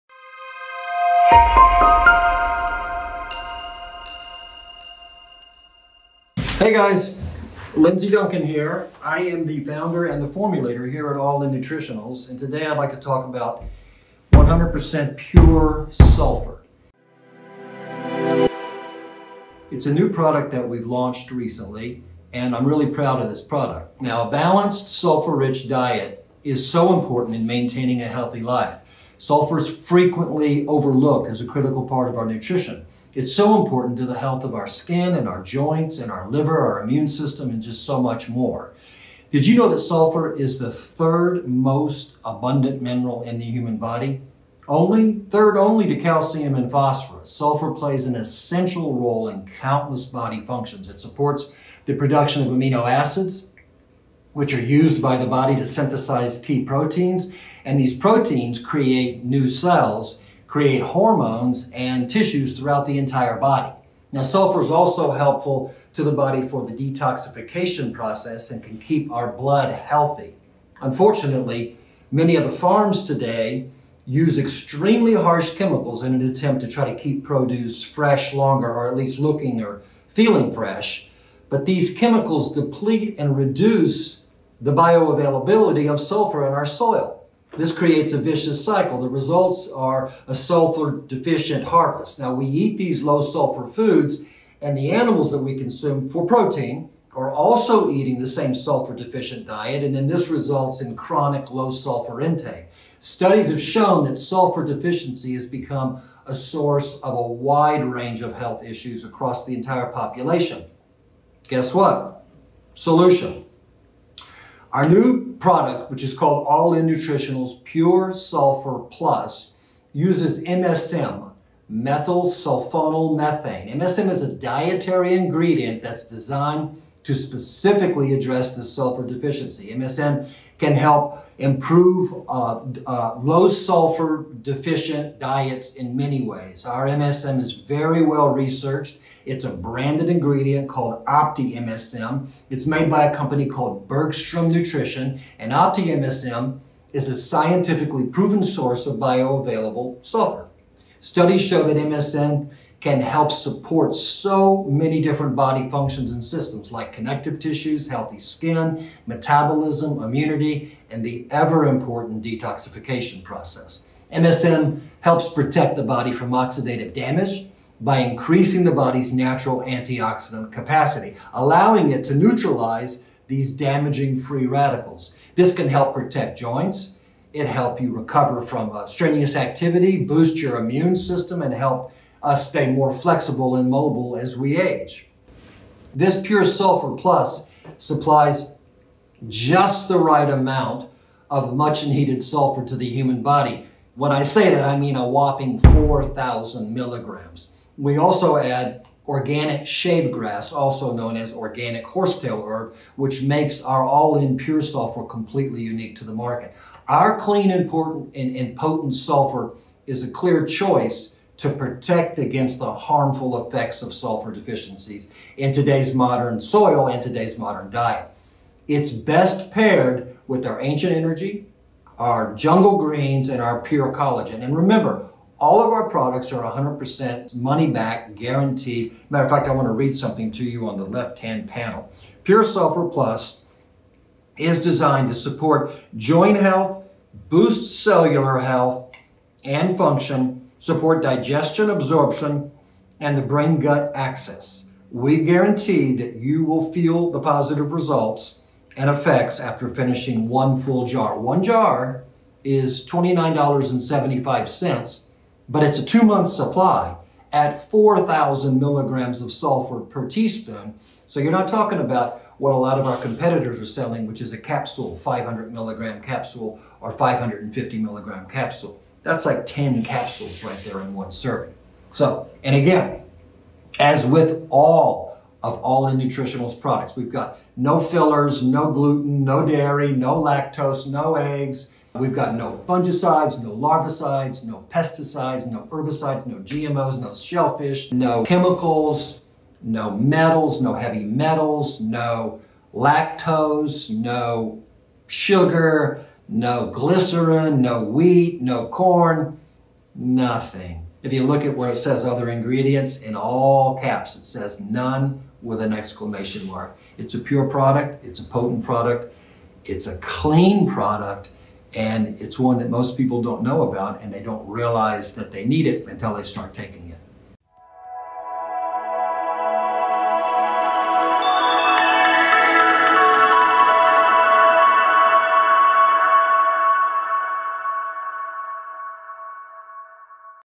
Sulfur (the forgotten nutrient) deficiency is a worldwide problem.
Are we getting enough sulfur in our diet?

A PubMed clinical study states "Sulfur, after calcium and phosphorus, is the most abundant mineral element found in our body. It is available to us in our diets, derived almost exclusively from proteins, and yet only 2 of the 20 amino acids normally present in proteins contains sulfur. One of these amino acids, methionine, cannot be synthesized by our bodies and therefore has to be supplied by the diet. Cysteine, another sulfur containing amino acid, and a large number of key metabolic intermediates essential for life, are synthesized by us, but the process requires a steady supply of sulfur."

Unfortunately, the sulfur content in our soil has been drastically depleted. "In 2015, the Sulfur Institute (TSI) estimated that less than half of the world's agricultural demand for sulfur was actually met – a global deficit of 12.5 million tonnes." There are many contributing factors causing sulfur deficiency in our soil, including the use of harsh chemicals in modern-day farming. Our plants (food) are therefore sulfur deficient, as are the animals (who eat said plants) which we consume for protein.

This forgotten nutrient is vital to many functions in the human body. Are you getting enough sulfur? The answer is likely, no.